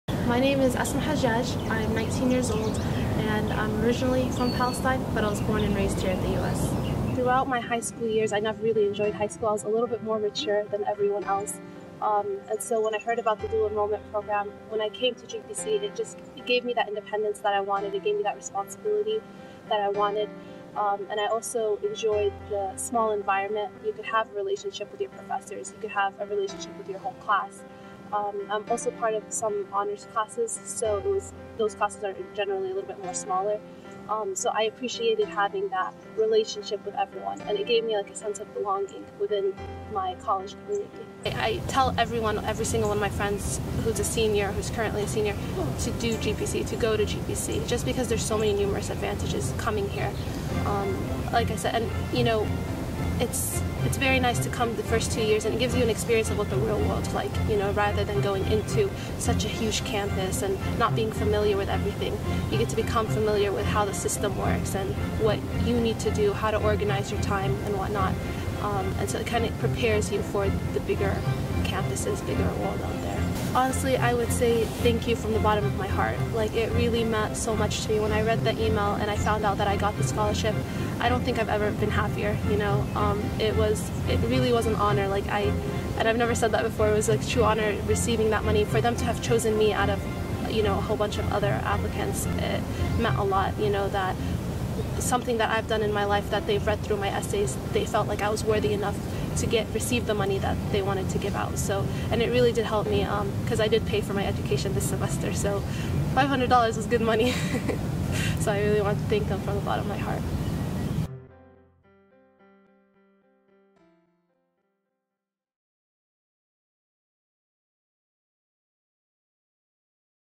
Asma Hajaj: 2013 GPC AA Scholarship Recipient
To support the GPC Alumni Association Scholarship Fund, visit giving.gpc.edu/alumnischolarship.
Asma Hajaj, a former dual enrollment student at Georgia Perimeter College explains how winning the Alumni Association Scholarship has made a difference in her education.